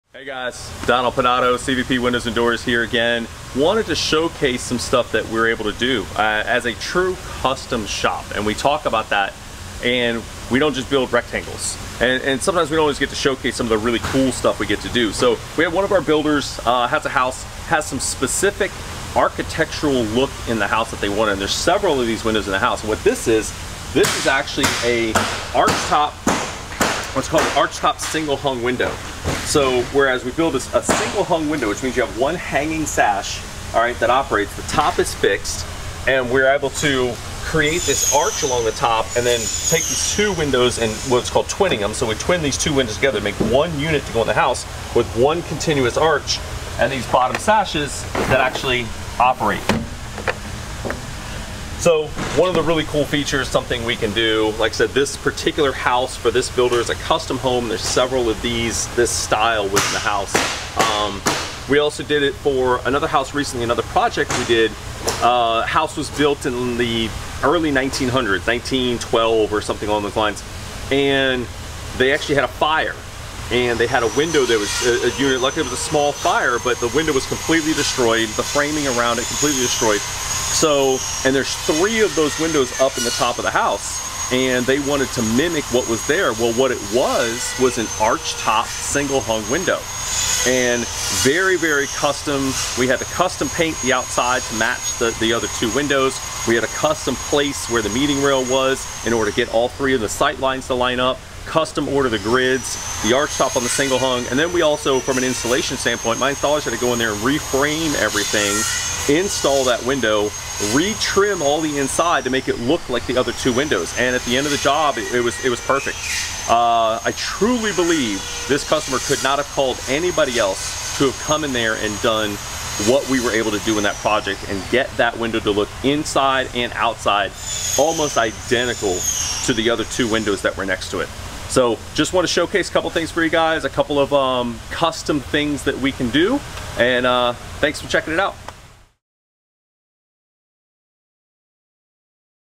Arch Top Single Hung Window: Custom Design Windows and Doors
Are you looking for custom design windows for your home? CVP Windows and Doors specializes in the design and manufacturing of custom windows and doors and might be the best fit for your needs!